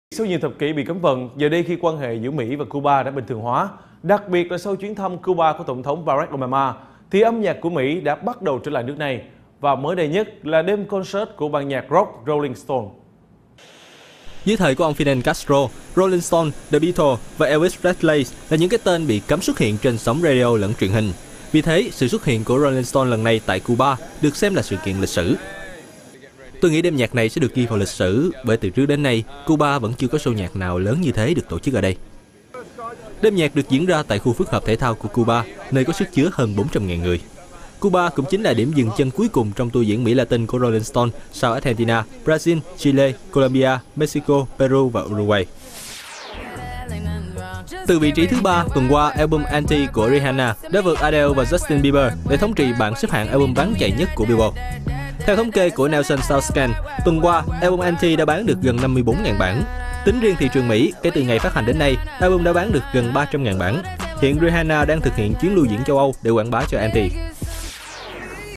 FBNC - Rolling Stone trở lại Cuba sau nhiều thập kỉ bị cấm
Sau nhiều thập kỉ bị cấm vận, giờ đây khi quan hệ giữa Mỹ - Cuba đã bình thường hoá, đặc biệt là sau chuyến thăm Cuba của Tổng thống Obama thì âm nhạc của Mỹ bắt đầu trở lại nước này. Và mới đây nhất là đêm concert của ban nhạc Rock Rolling Stones.